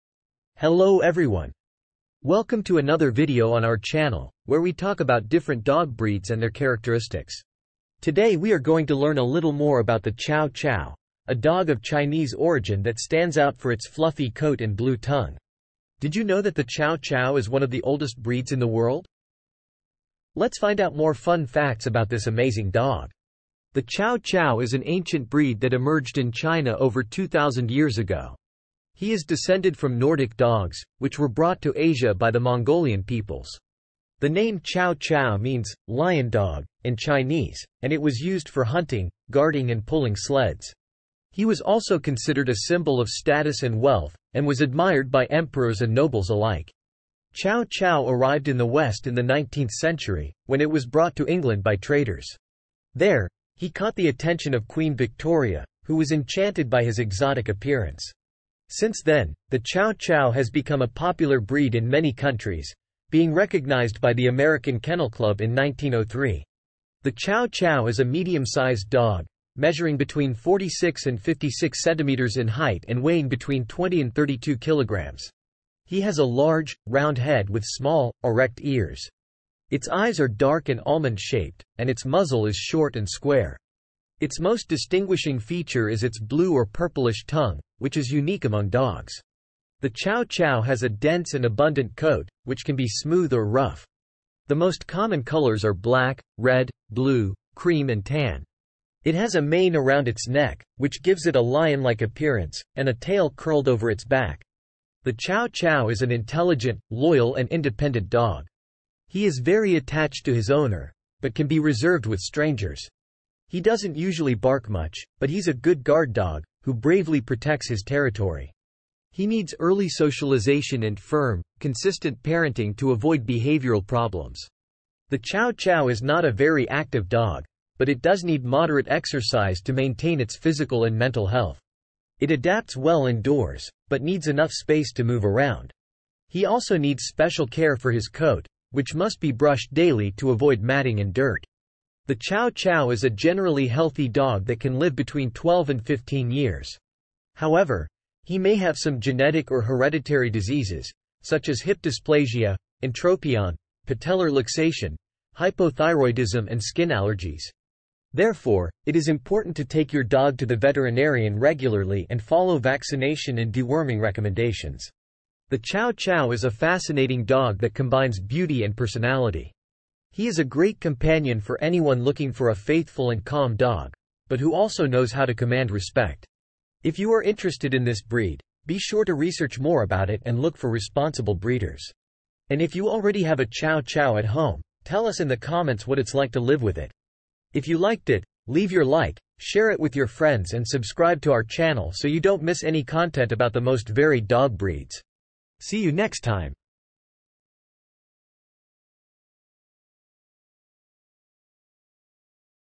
Everything you need to know about the Chow Chow, the oldest breed in the world!
Hello everyone!
 Today we are going to learn a little more about the Chow Chow, a dog of Chinese origin that stands out for its fluffy coat and blue tongue. Did you know that the Chow Chow is one of the oldest breeds in the world? Let's find out more fun facts about this amazing dog!